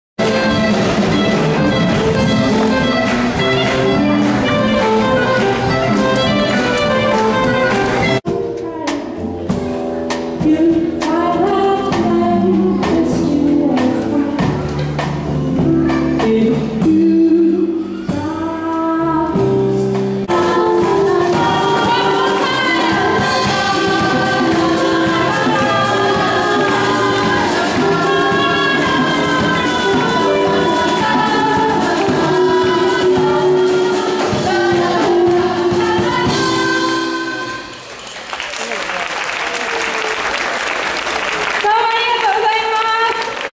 小樽初 0歳からのジャズライブ！
子育て中の家族が気軽に聴ける「0歳からのジャズライブ」が、6月
24日(土)10:30から、いなきたコミュニティセンター(稲穂5)4階体育室で開かれ、小樽近郊に住む子ども連れの家族約210名が参加し、プロのジャズをライブで楽しんだ。